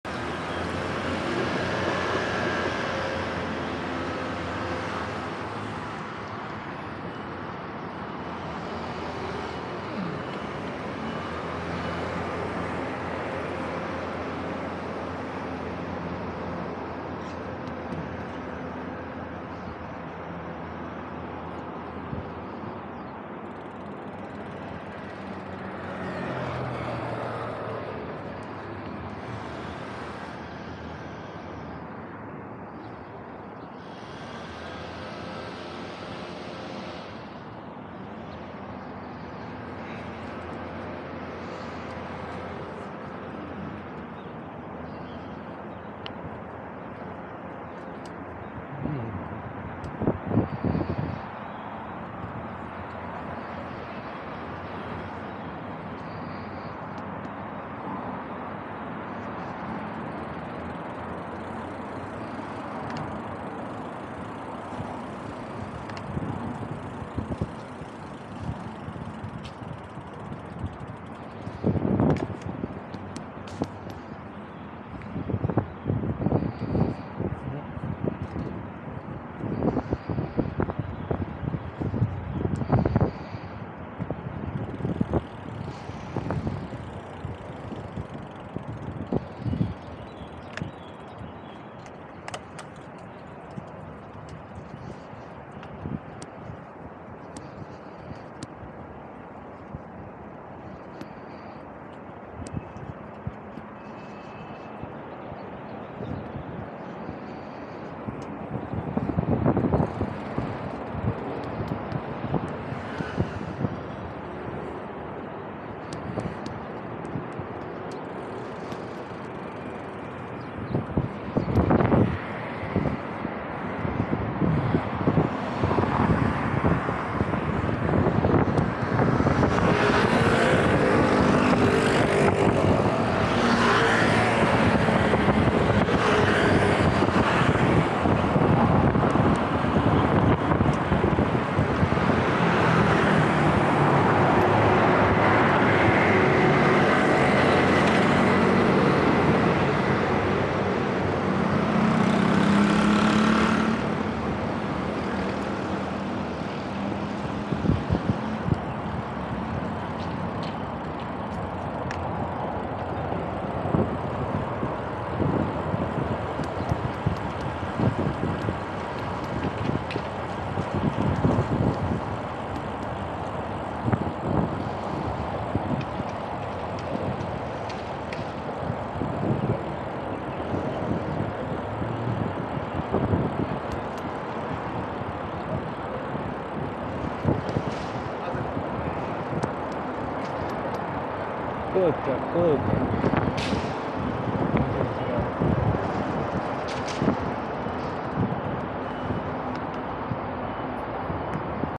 Insta360GO2 初體驗。單車如何跨越基隆河(1)：中山橋
中山橋跨越基隆河，連接南岸的圓山與北岸的劍潭山
橋南為中山北路三段，北側為中山北路四段。
這是2007年10月完工通車的 中山新橋 。
至於 中山舊橋 於2002年12月20日拆橋遷建，但遷建處仍未定，舊橋拆除後切為435塊，堆在再春游泳池原址至今。